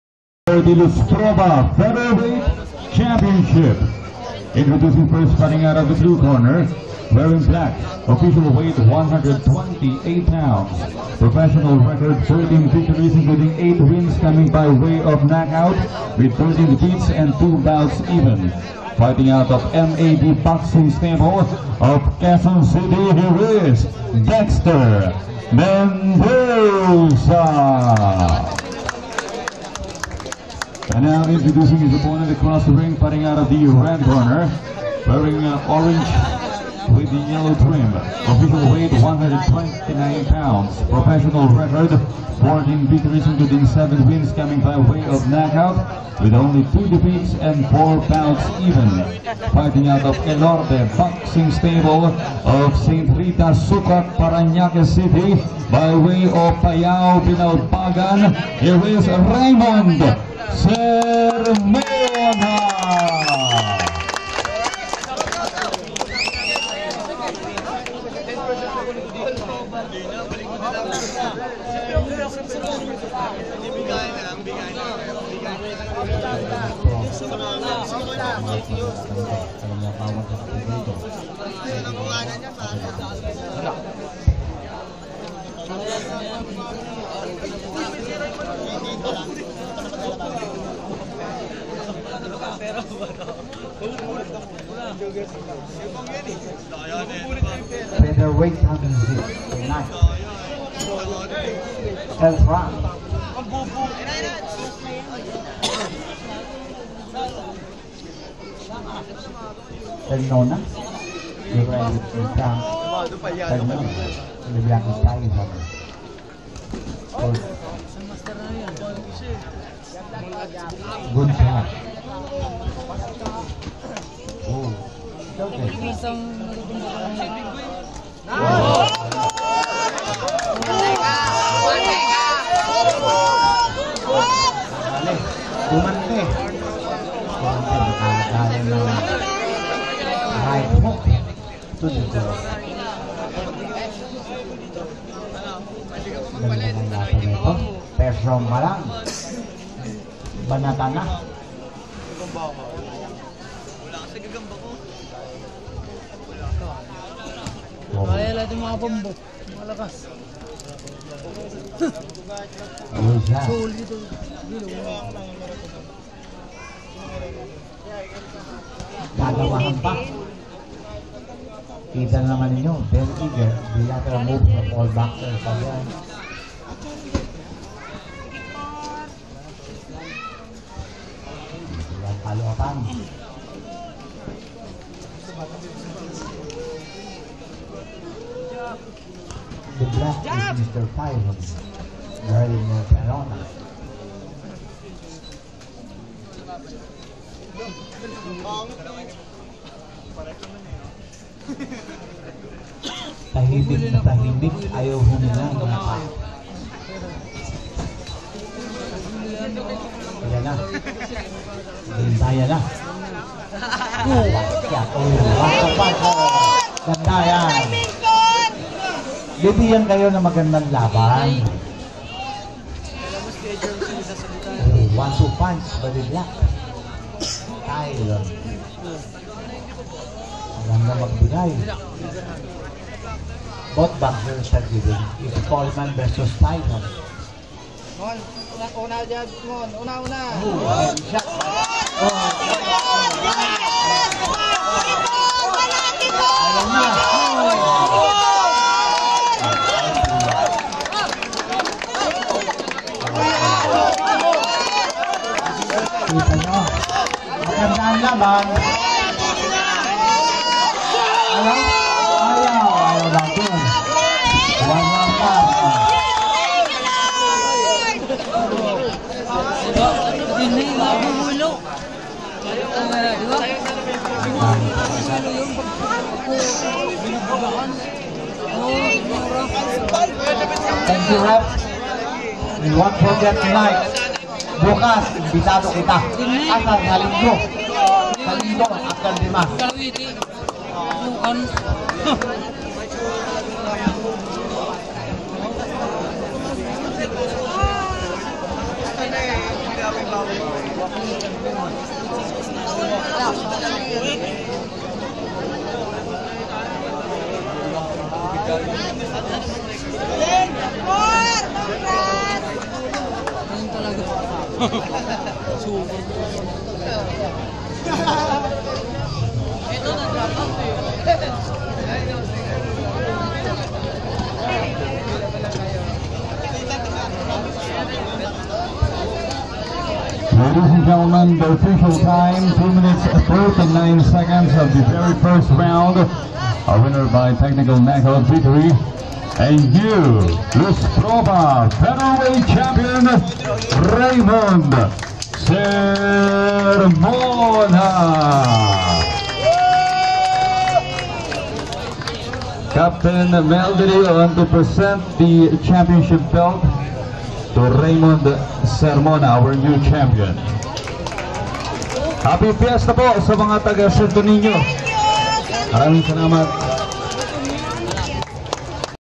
raymond sermona vs dexter mendoza championship
sto. nino paranaque city